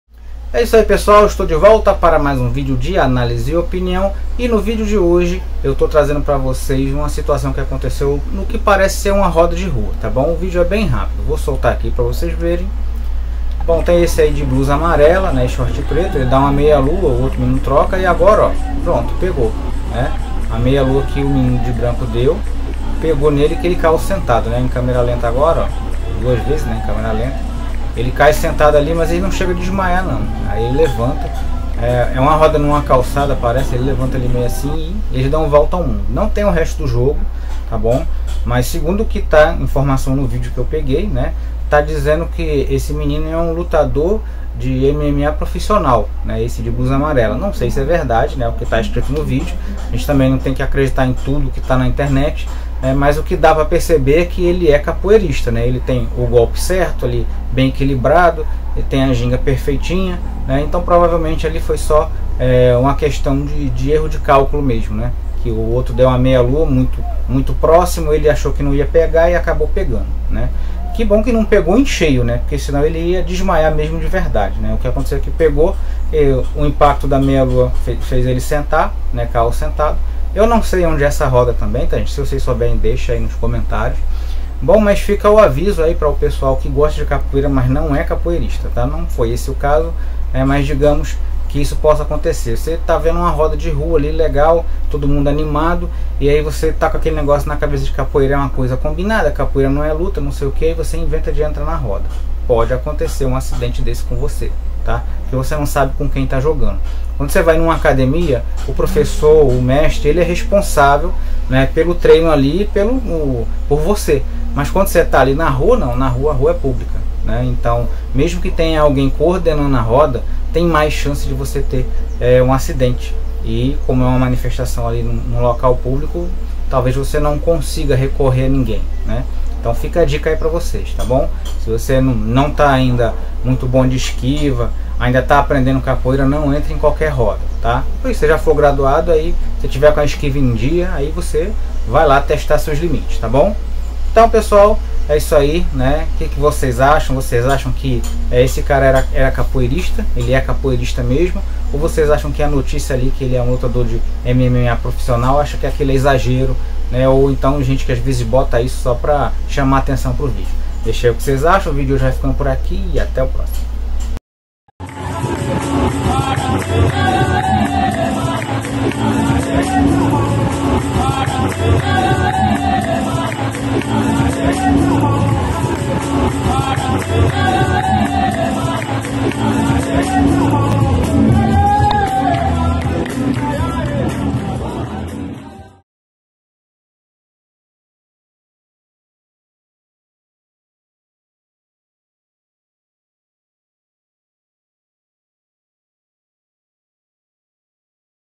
Jogador não esquivou no tempo e a Meia-lua acertou de leve mas derrubou o outro jogador
CANAIS QUE RECOMENDO
VRT INFO TEC: